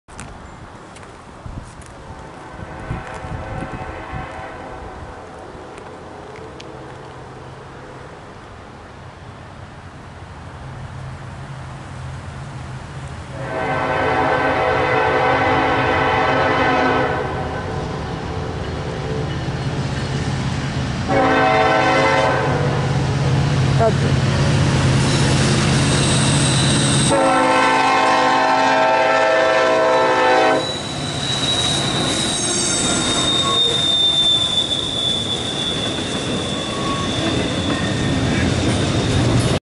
NS Patched Sd70ACe #1042 Leads and Intermodal Through Tyrone, PA
While I was waiting for NS 8101 which wouldn't come, I caught something pretty interesting. NS 1042 (a patched Sd70Ace) was leading. I'm not sure why the left side was patched, but that makes this catch all the more special.